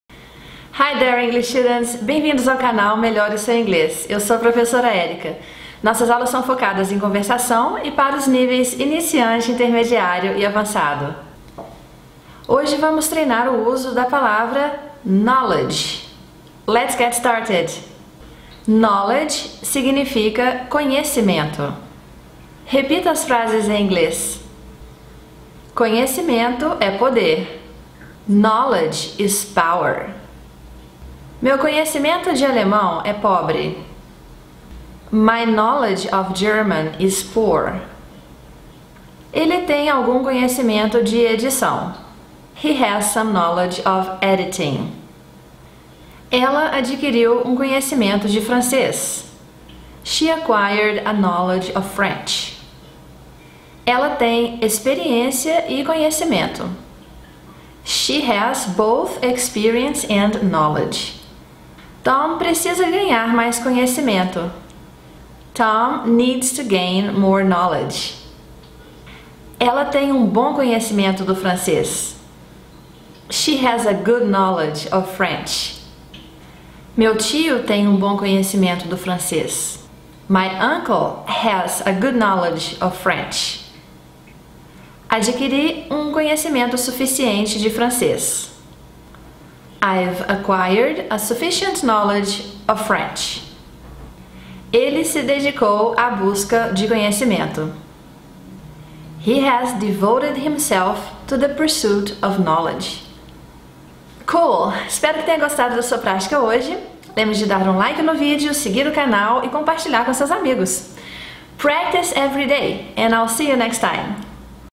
PRONUNCIATION: Como pronunciar a palavra KNOWLEDGE!!! | Profa. Érika de Pádua
Tema da VIDEO AULA de Inglês de hoje:
Como pronunciar a palavra KNOWLEDGE!!!